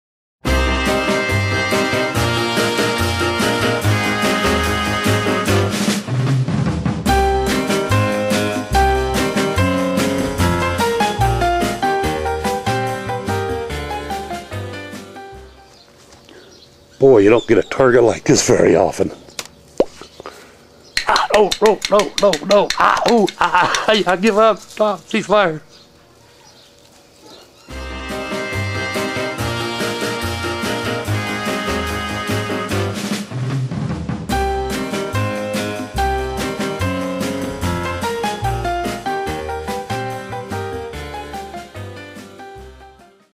Let's go hunting!
How to hunt around the house - use a rubber band gun!